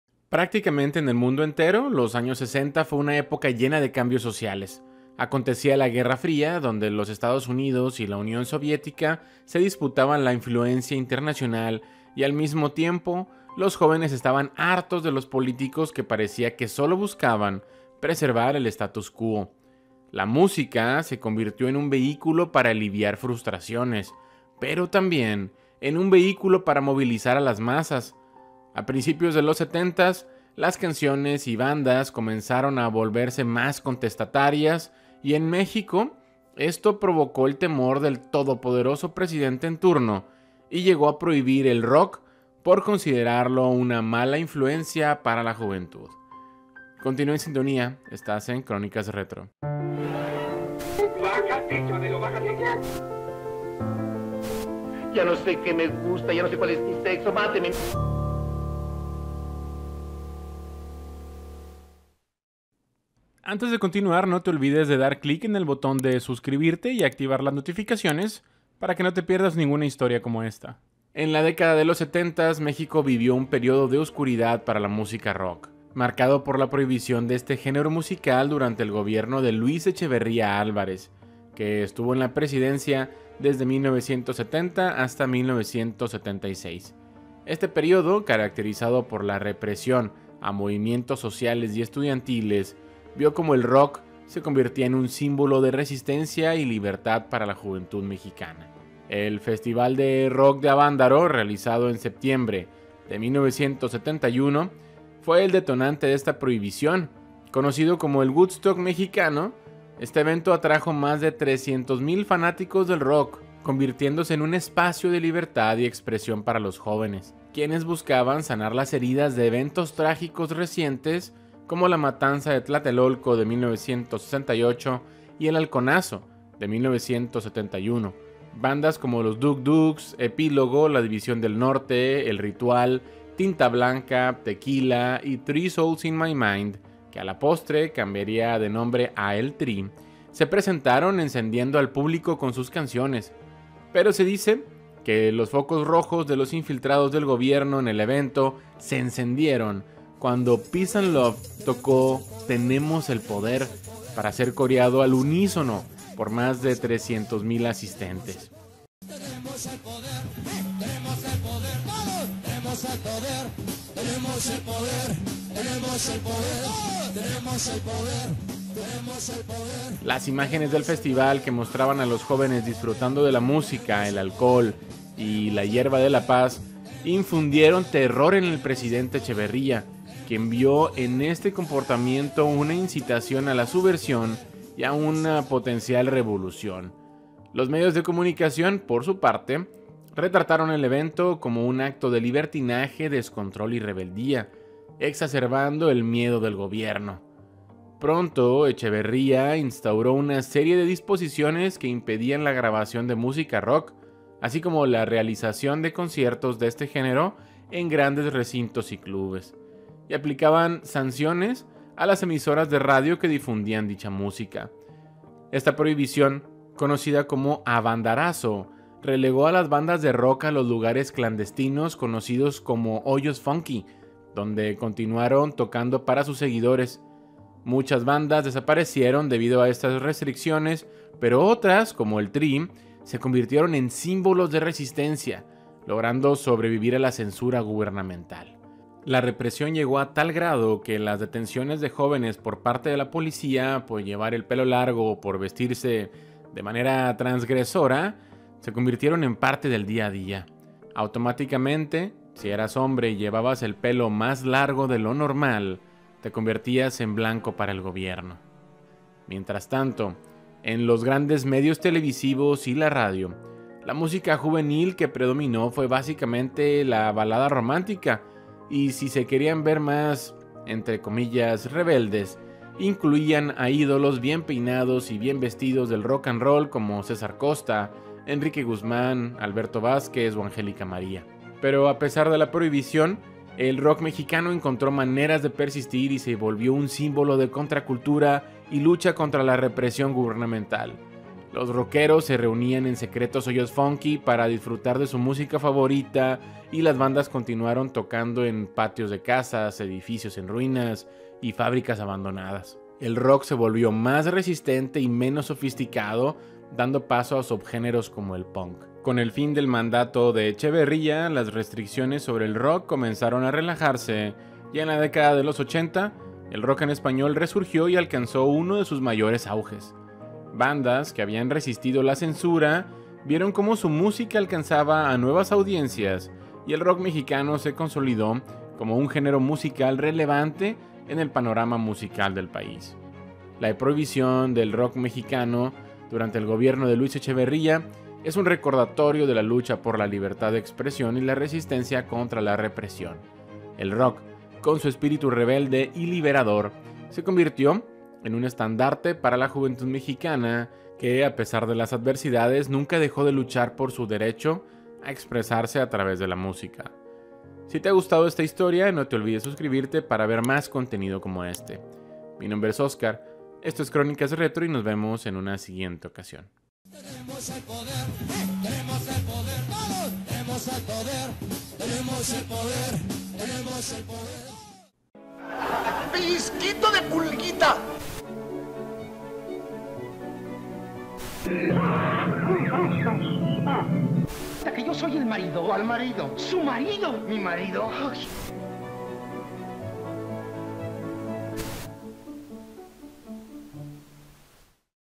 Avándaro y la muerte del Rock Mexicano
Descubre la historia detrás del Festival Rock y Ruedas de Avándaro, el evento que desencadenó la prohibición del rock en el país y cómo los amantes de este género musical lucharon por mantenerlo vivo en la clandestinidad. 🚀🔥